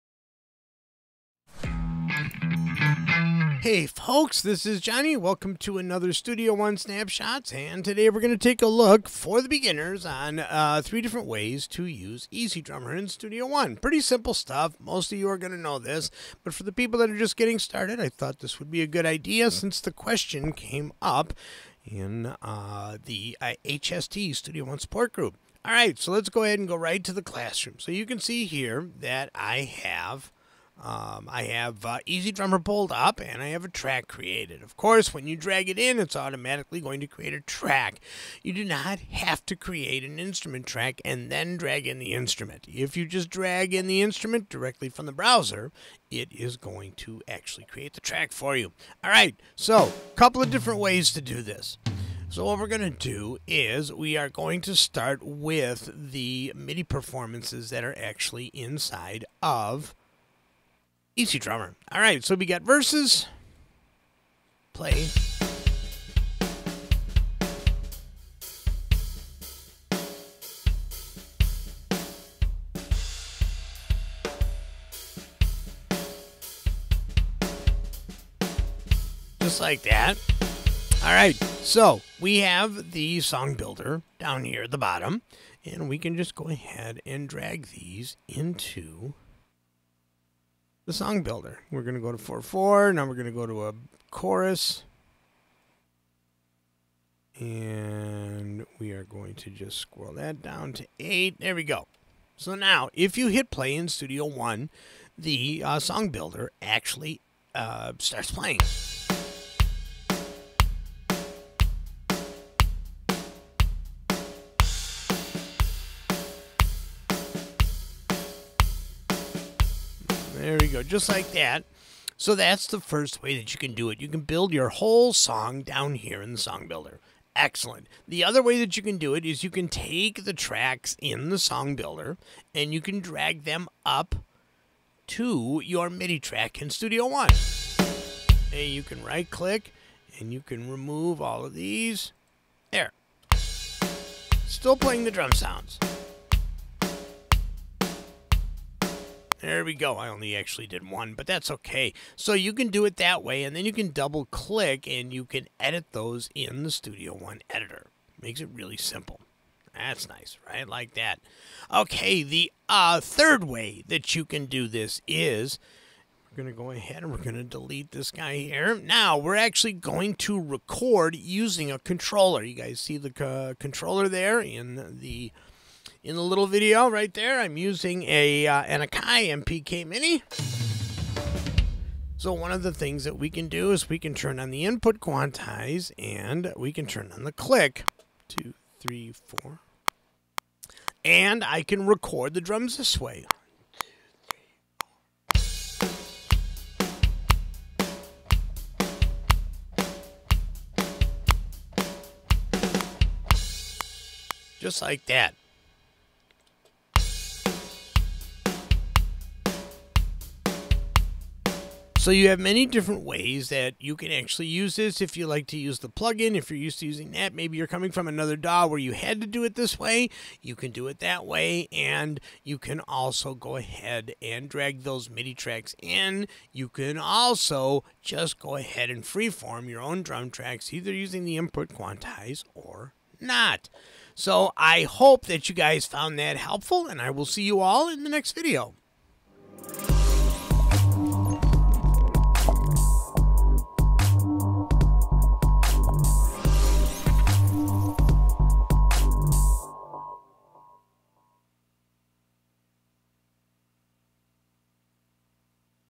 3 Ways To Use EZDrummer In Studio One Pro 7 - Studio One Snapshots
In this video, I show 3 easy ways to use EZDrummer in Studio One Pro 7 for songwriting and editing.